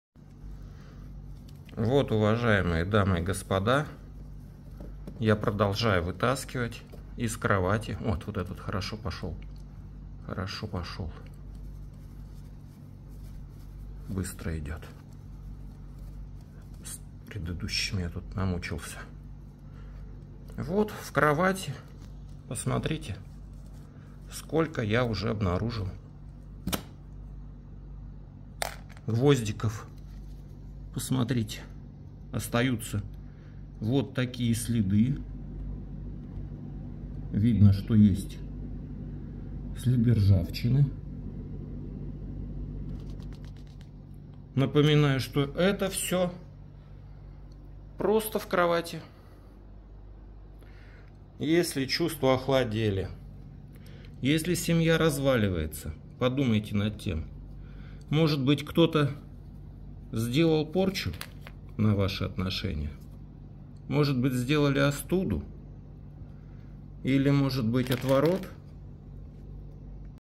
ОСТУДА , ОТВОРОТ, РАССОРКА
Никогда не думал что увижу такое количество гвоздей вбитых в кровать. Это просто жуть. Когда мне сказали что кто то , что то сделал на кровать и там много гвоздей я подумал 3,4,6  но когда увидел этот подклад  я поверить не мог своим глазам. И думаю вряд ли такое ещё раз увижу.Каждый гвоздь вбит с ненавистью и злобой. Диструктивная энергетика чувствуется сразу как только переступить порог комнаты.
Если у вас проблемы в здоровье, делах и отношениях звоните мне я обязательно помогу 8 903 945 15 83 ватсап Алексей Юрьевич